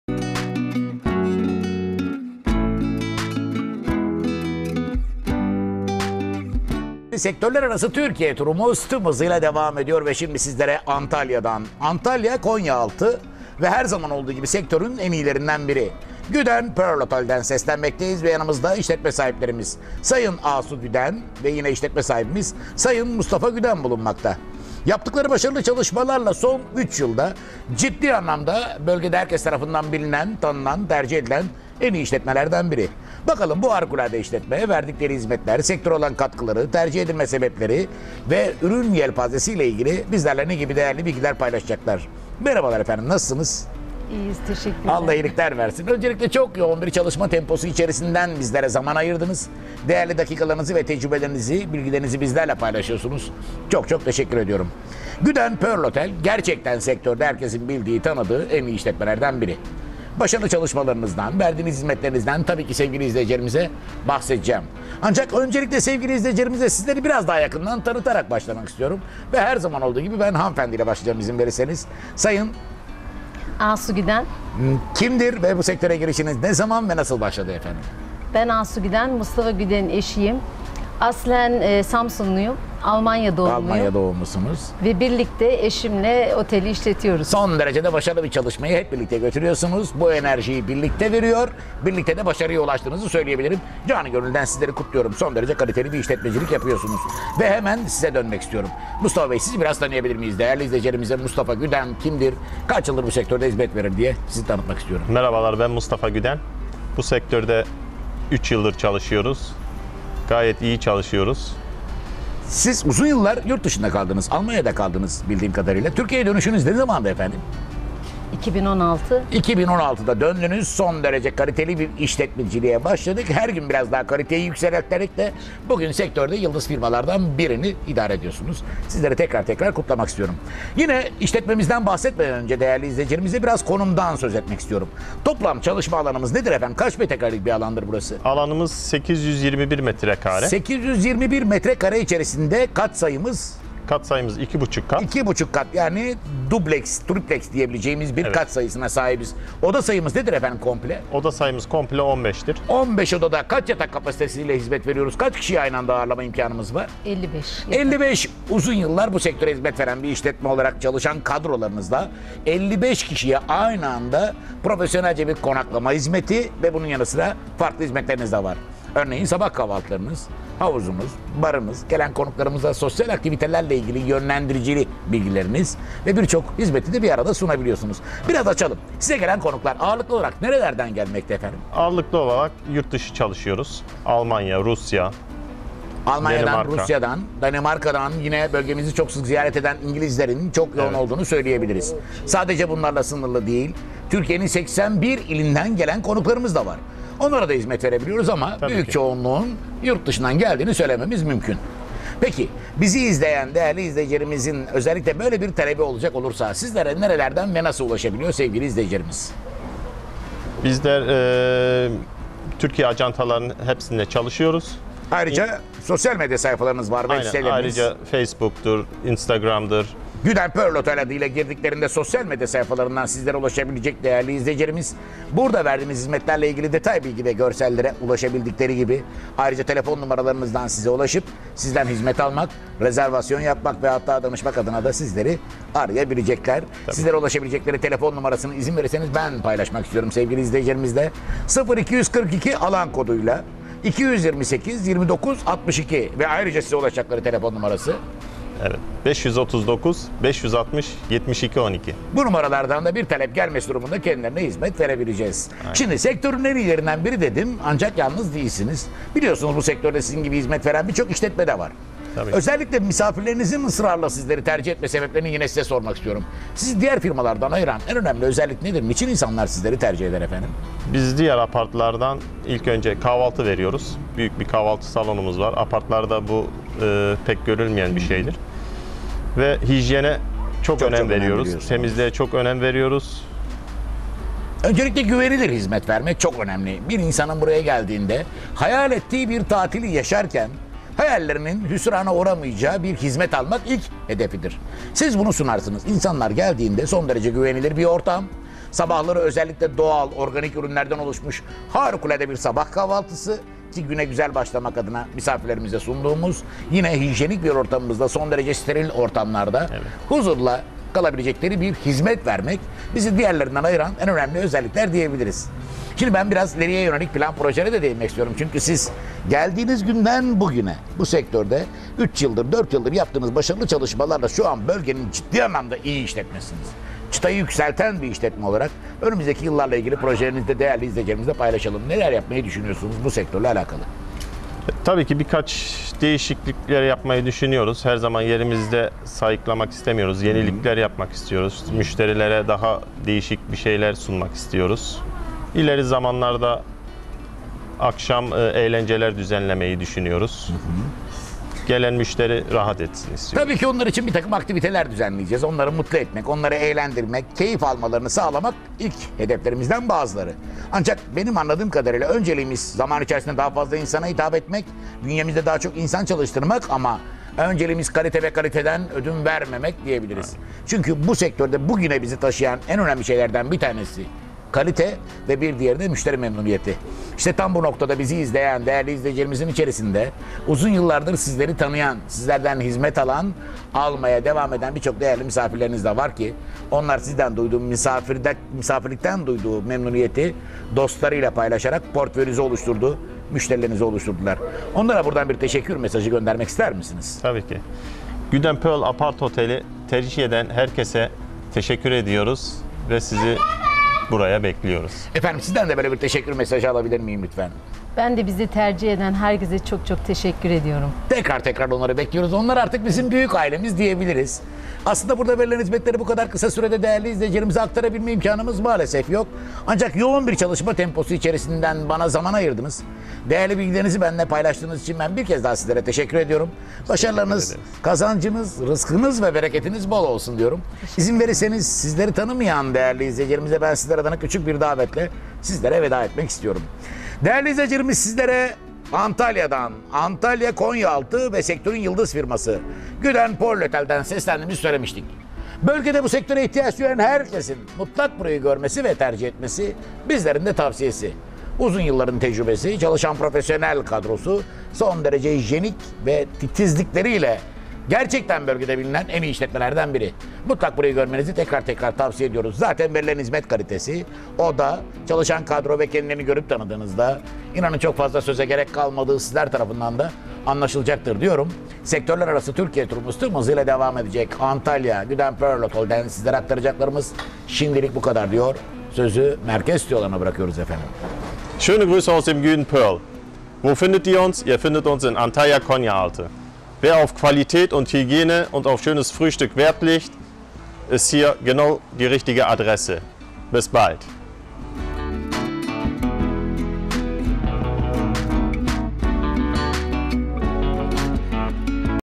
KALİTELİ KONAKLAMA | GÜDEN PEARL APART OTEL
Güden Pearl Apart Otel Firmasının Sahipleri Asu Güden ve Mustafa Güden vermiş oldukları hizmetler hakkında Posta Kobi’ye bilgi verdi.